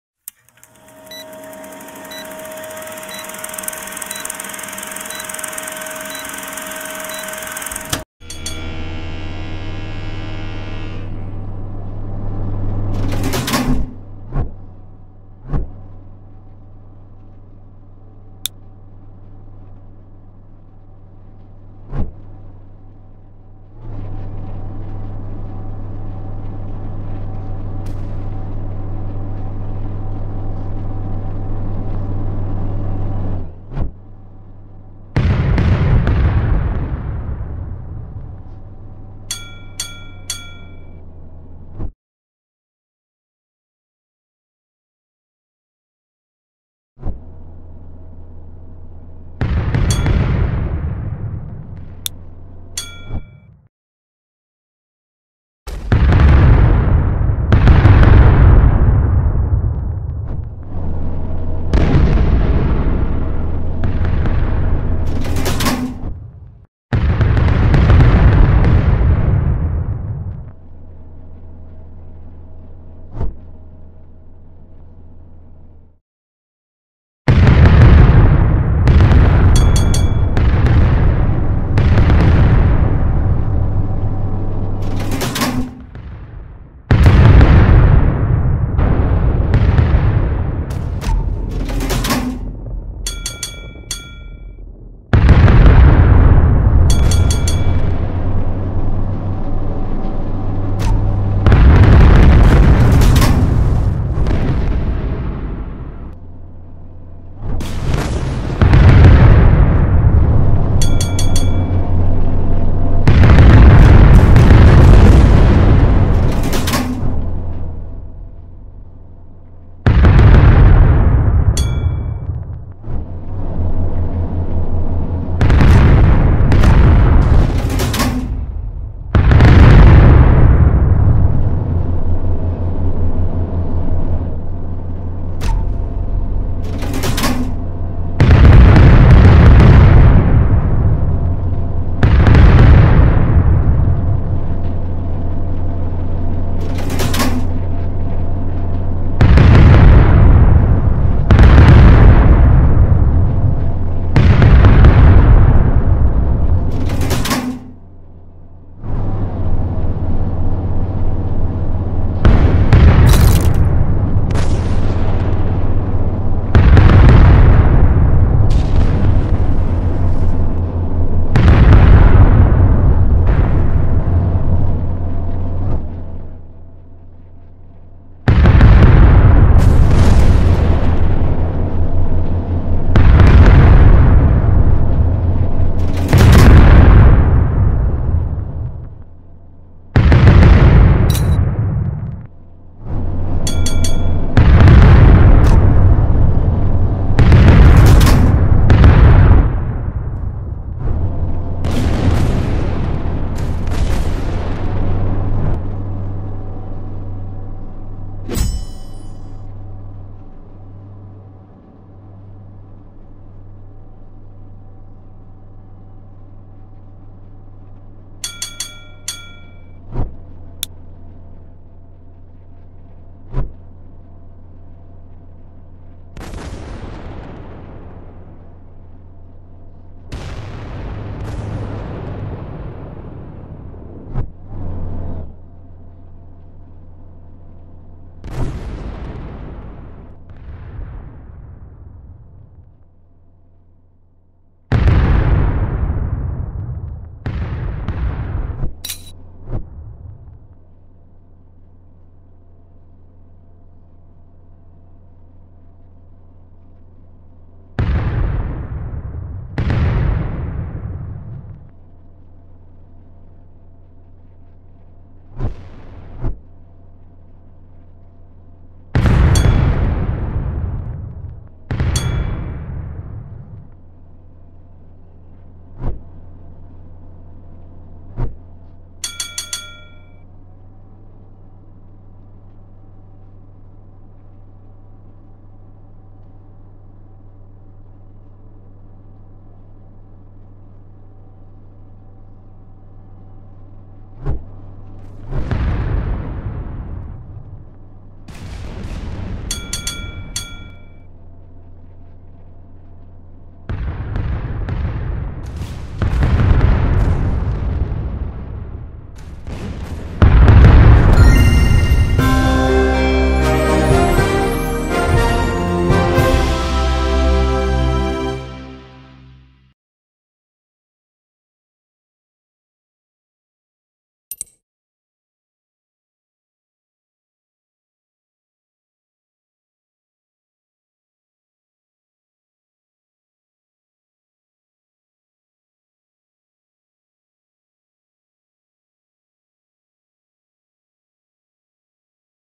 KMS Tirpitz, you may not get any kills, but you give great damage assistance.
In this video, I use KMS Tirpitz to assist my teammates in sinking the enemy. Will my team win? or will we get distroyed?.  Watch until the end to find out!!. Have a nice day. :).